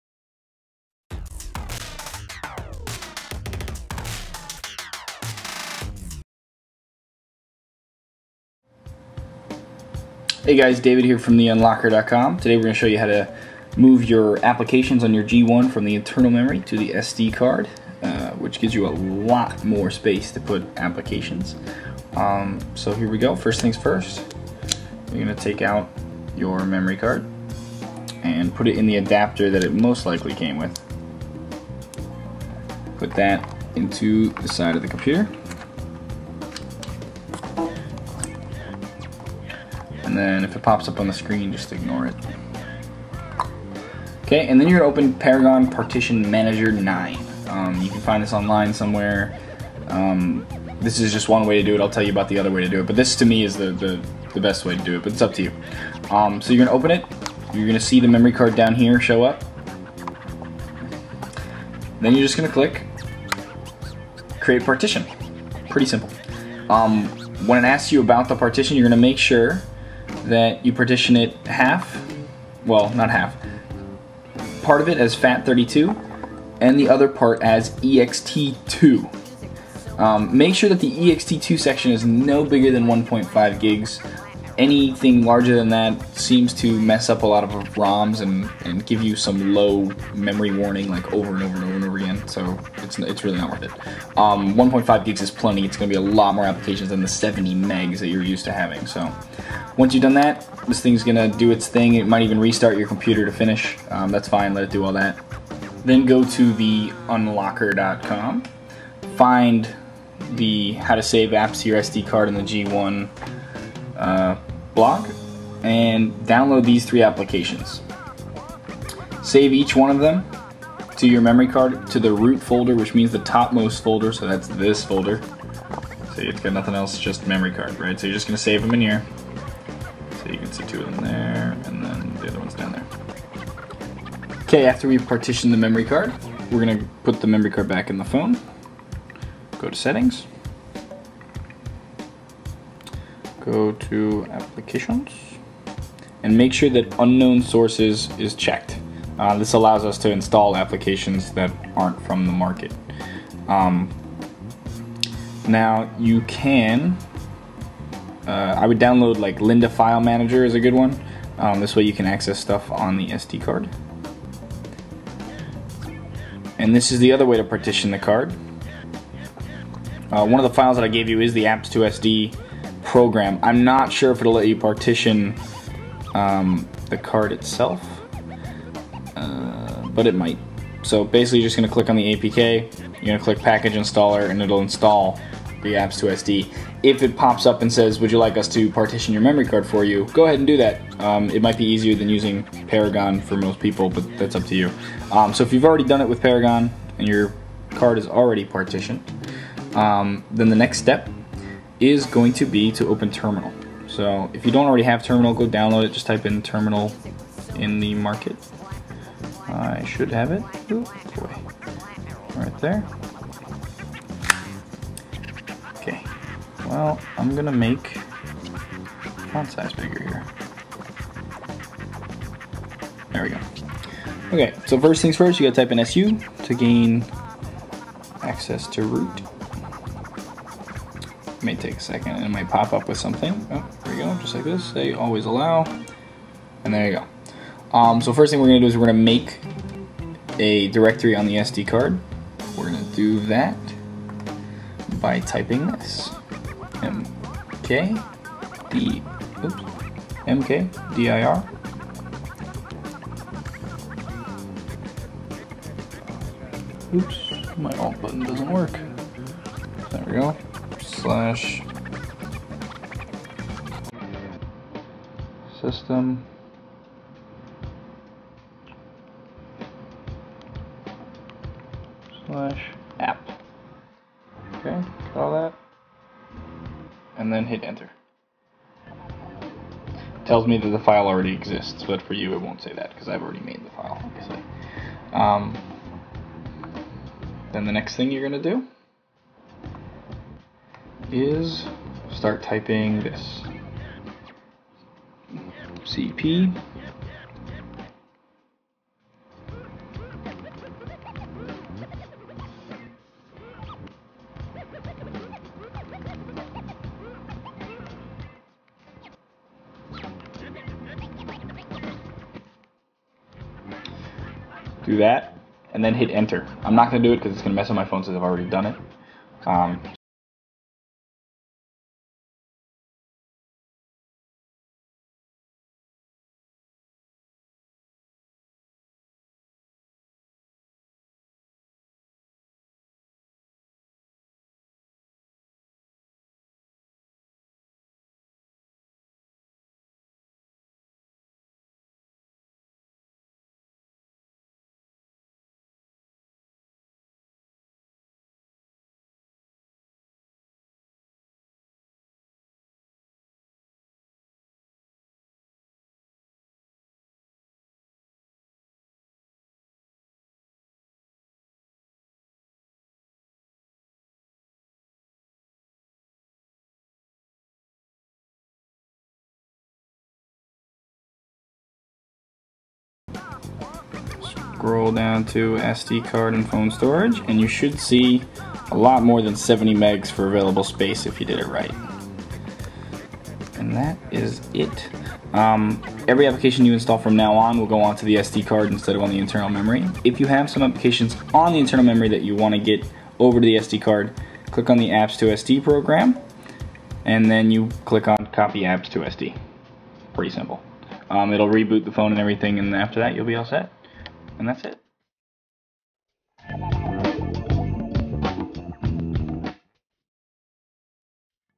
How To - Apps2SD for the G1
We show you how to get the applications from your internal memory to the much larger SD card so you can have a lot more of your favorite applications!